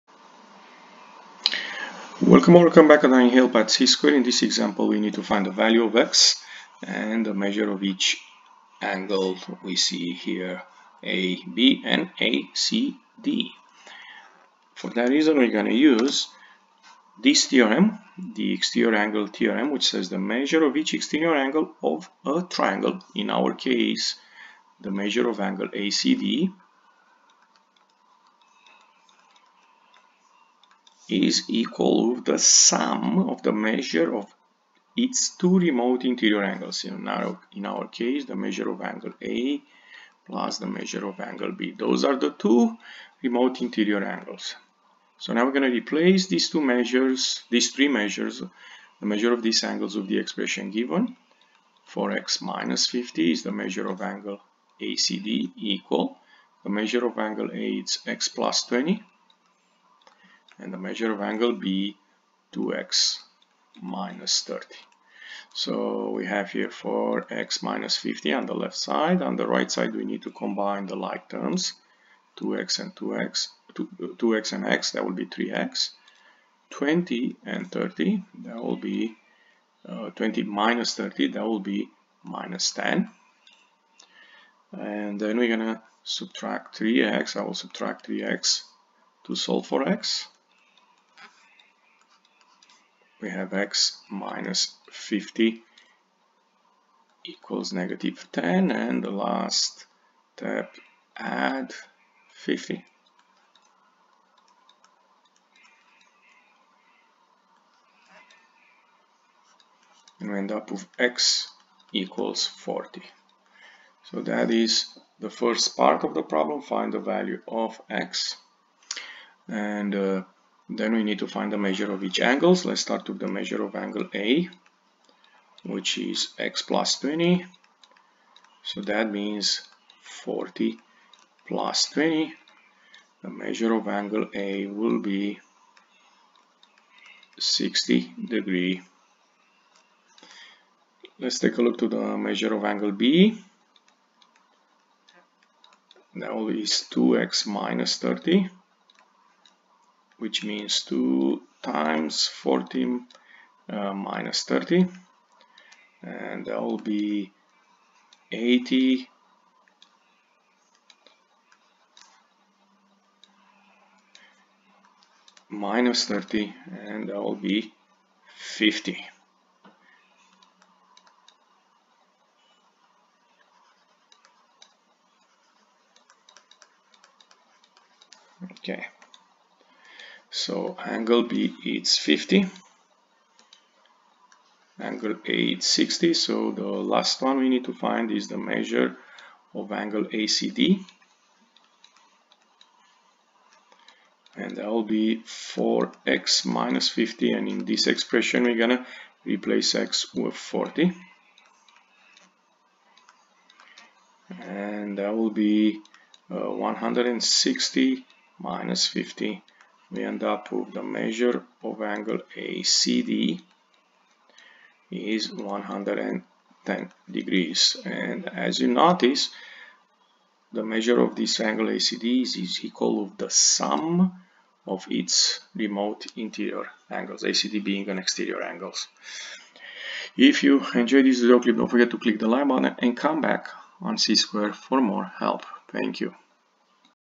INH: Triangle: Exterior Angle Theorem #3
This example covers finding the exterior angle of a triangle using two methods.